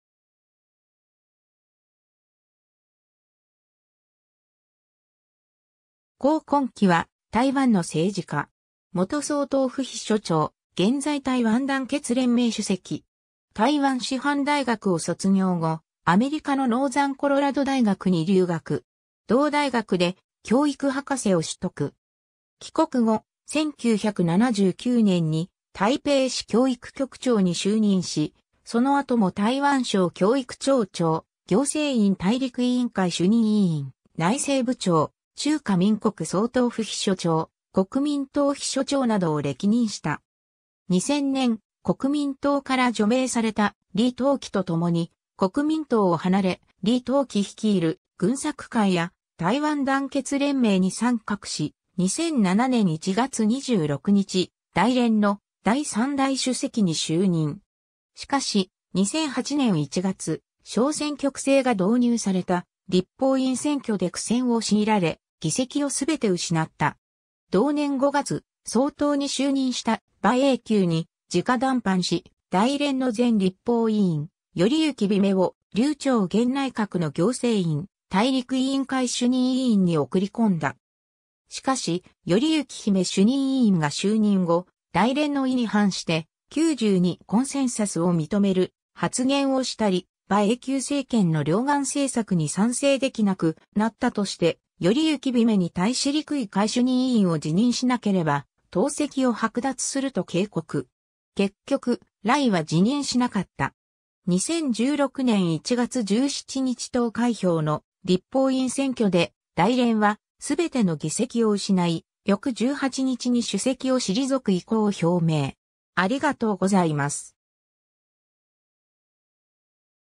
黄昆輝
黄 昆輝（こう こんき）は、台湾の政治家。
元総統府秘書長、現在台湾団結連盟（台連）主席。
 台湾師範大学を卒業後、アメリカのノーザンコロラド大学に留学。
同大学で教育博士を取得。
帰国後、1979年に台北市教育局長に就任し、その後も台湾省教育庁長、行政院大陸委員会主任委員、内政部長、中華民国総統府秘書長、国民党秘書長などを歴任した。
 2000年、国民党から除名された李登輝とともに国民党を離れ、李登輝率いる「群策会」や「台湾団結連盟」に参画し、2007年1月26日、台連の第3代主席に就任。
しかし、2008年1月、小選挙区制が導入された立法委員選挙で苦戦を強いられ、議席を全て失った。
同年5月、総統に就任した馬英九に直談判し、台連の前立法委員頼幸媛を劉兆玄内閣の行政院大陸委員会（陸委会）主任委員に送り込んだ。
しかし、頼幸媛主任委員が就任後、台連の意に反して「92コンセンサス」を認める発言をしたり、馬英九政権の両岸政策（対中政策）に賛成できなくなったとして、頼幸媛に対し陸委会主任委員を辞任しなければ党籍を剥奪すると警告。
結局、頼は辞任しなかった。
 2016年1月17日投開票の立法委員選挙で台連は全ての議席を失い、翌18日に主席を退く意向を表明。